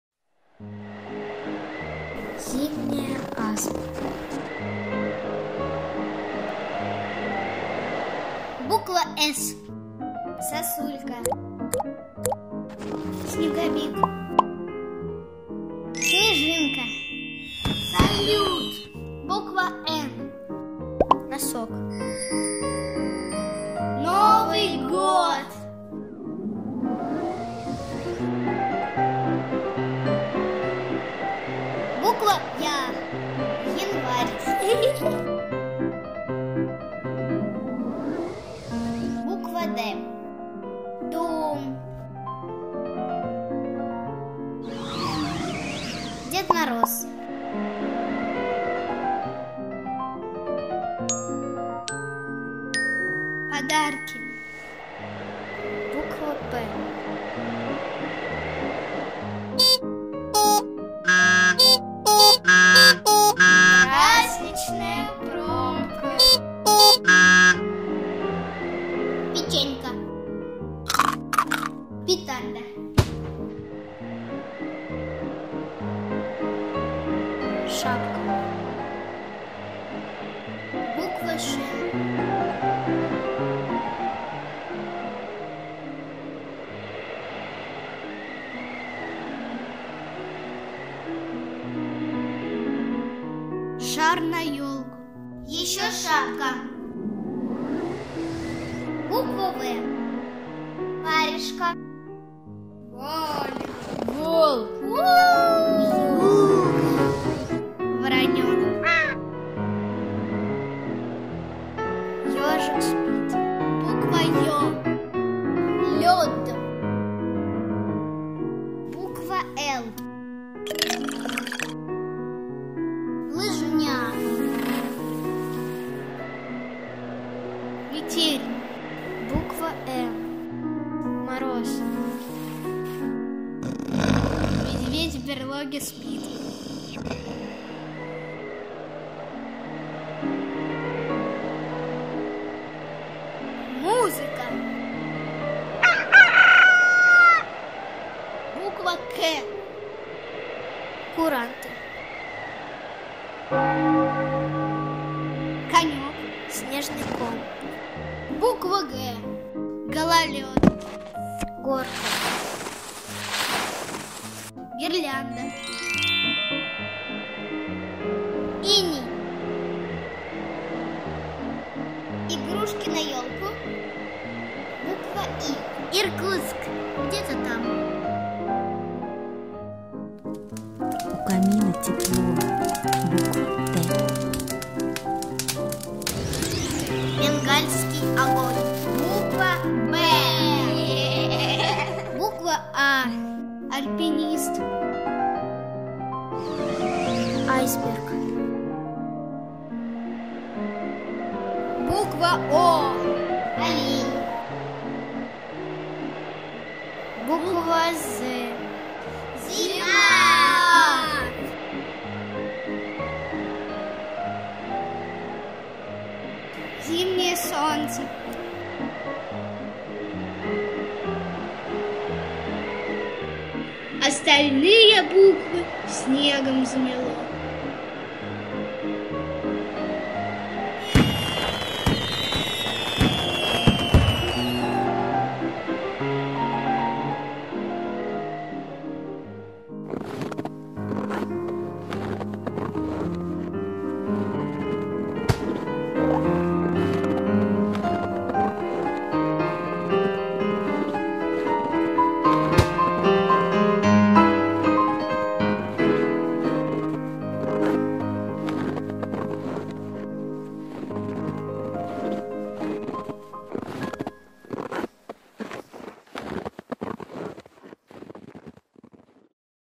"Зимняя Азбука"
Фильм снятый в рамках детского зимнего анимационного лагеря